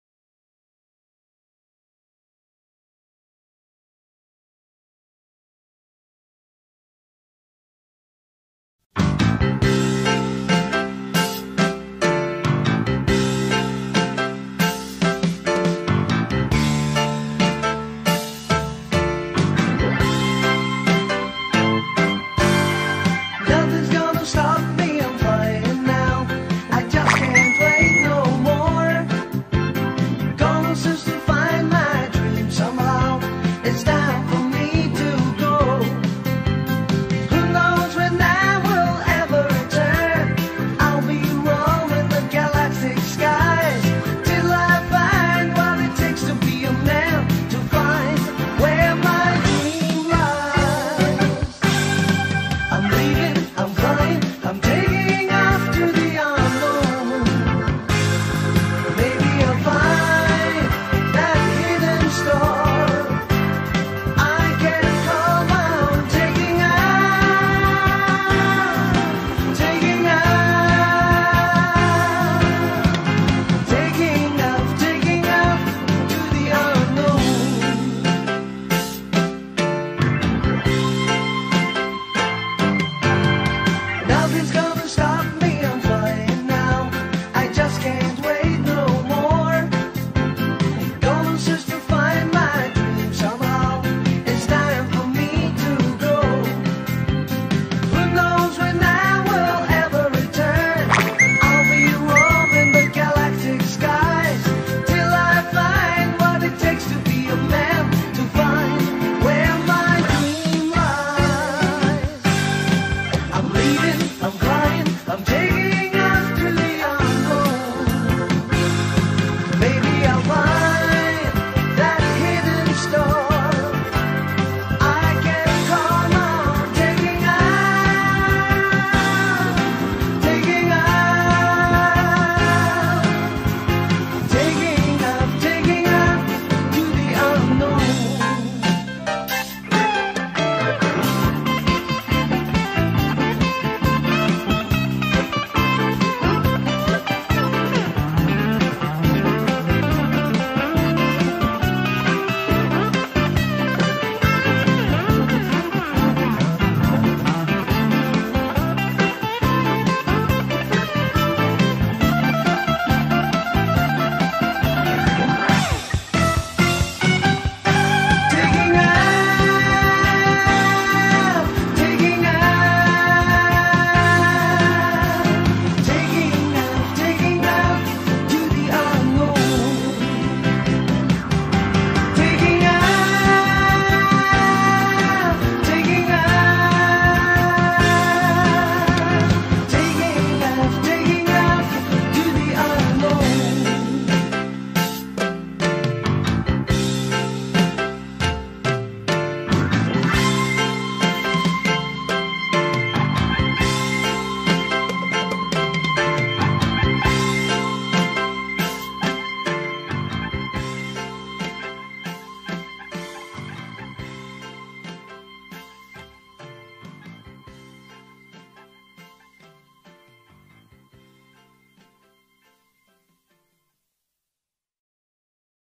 【Nゲージ】銀河鉄道888 Galaxy Express 888 アルデバラード8号【Ver2.0】
補助動力を追加して走行を安定させました。
編成数も増えています。